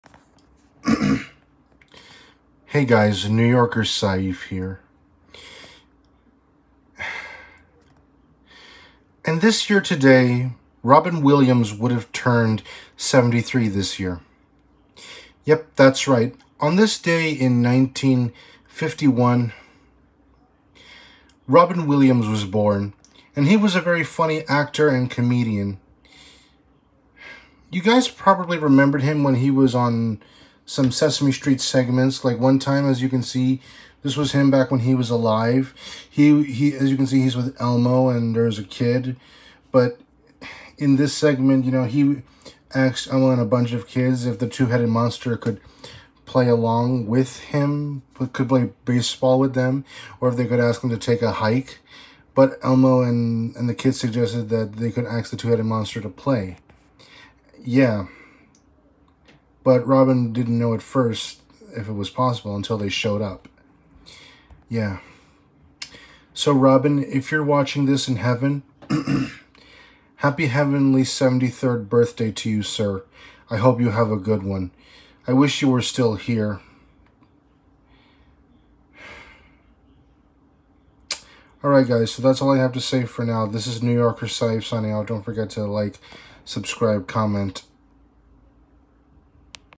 Today would have been Robin Williams’s 73rd birthday.
On this day in 1951, Robin Williams was born. Happy heavenly 73rd birthday.￼